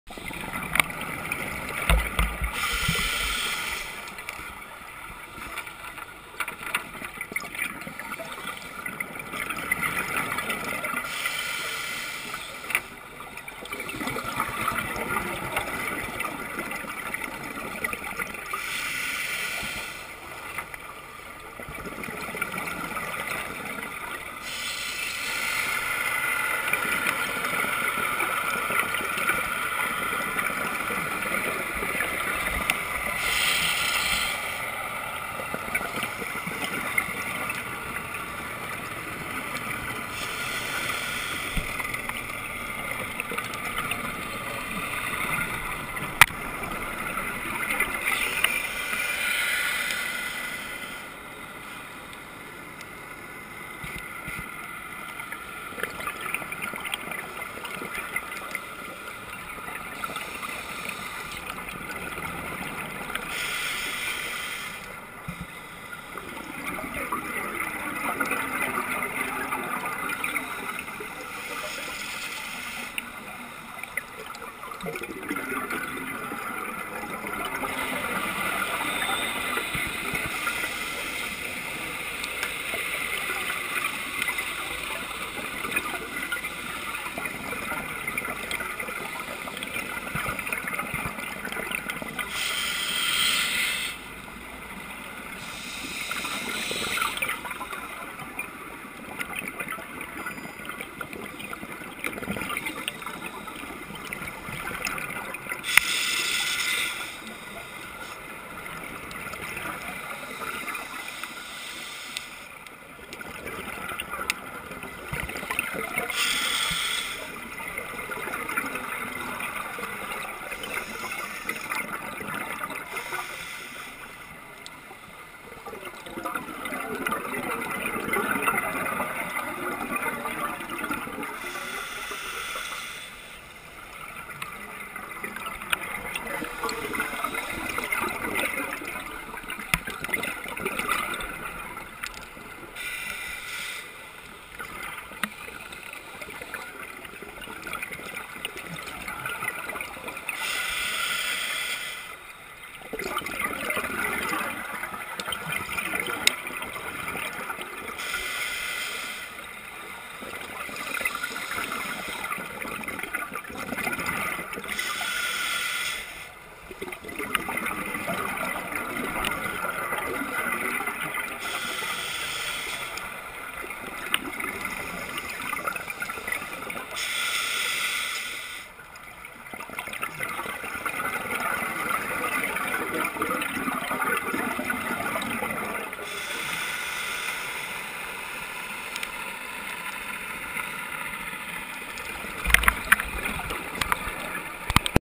Diving the MV Trident in Thailand
The MV Trident was an old tech diving live aboard vessel that was purpose sunk for diving, purposely sunk 35 meters down!